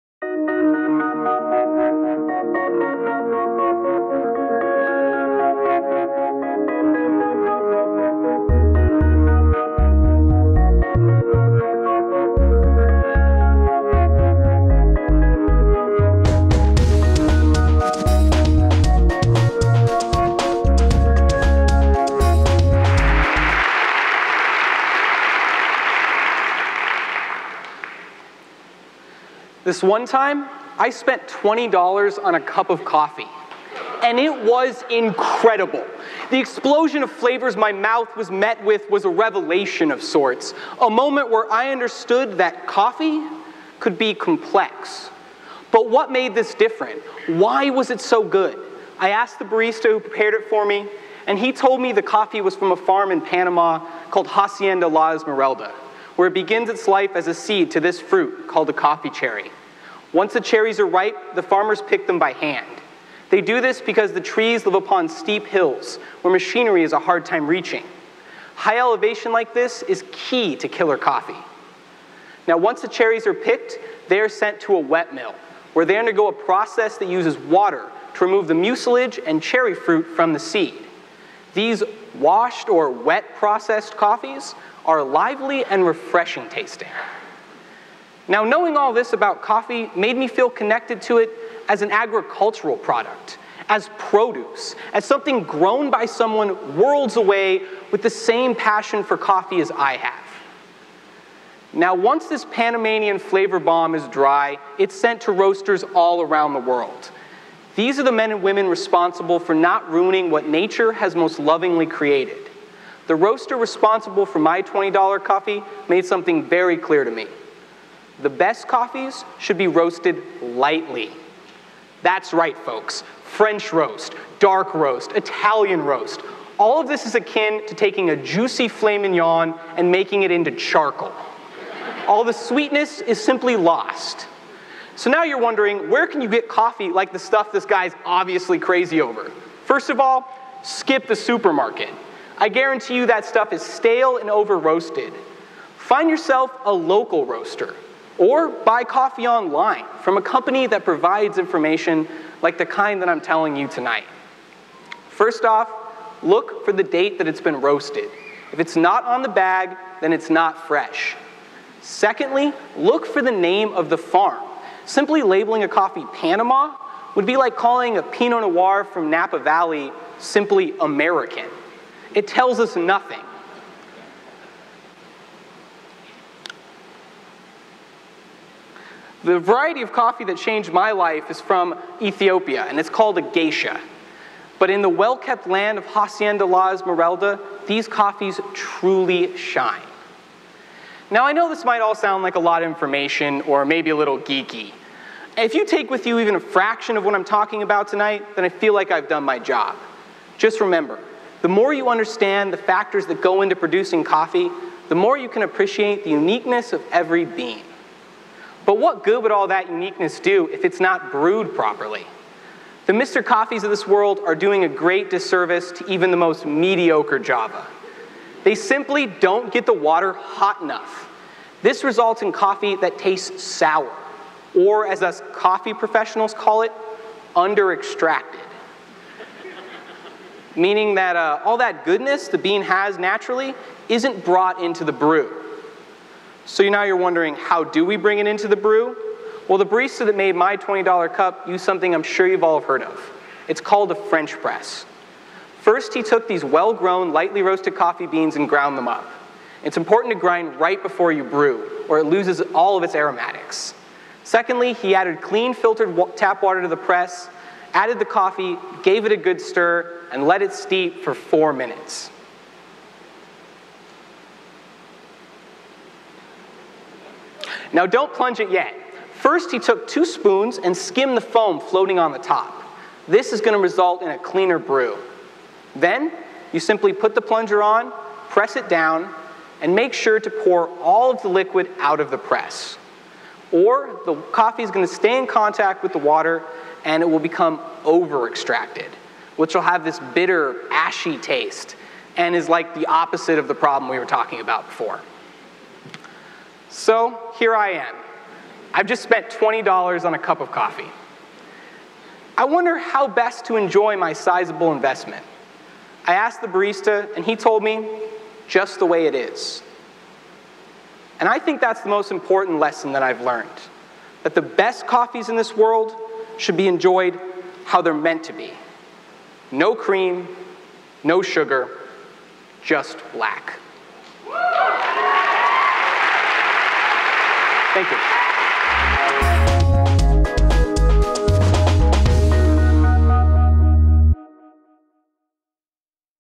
Coffee Is The New Black | Ignite Phoenix #14
Remember a time when all coffee cost 50 cents and tasted like dirt? For the past decade, a coffee renaissance has led to roasteries in every major city, cafes with new brewing methods, and coffee farmers sharing the spotlight. If this all sounds foreign to you, let Barista Bill explain and ensure no one in the audience sleeps well tonight!

Presenter: Bill Kennedy

Biography: Bill Kennedy is an artist, an engineer, and a scientist; or more simply put, Bill is a barista at a specialty coffee shop. He studied theatre at ASU before moving to Portland, Oregon, where he once spent twenty bucks on a cup of coffee and loved it. In 2010, Bill returned to Phoenix to share his new religion: the gospel of java.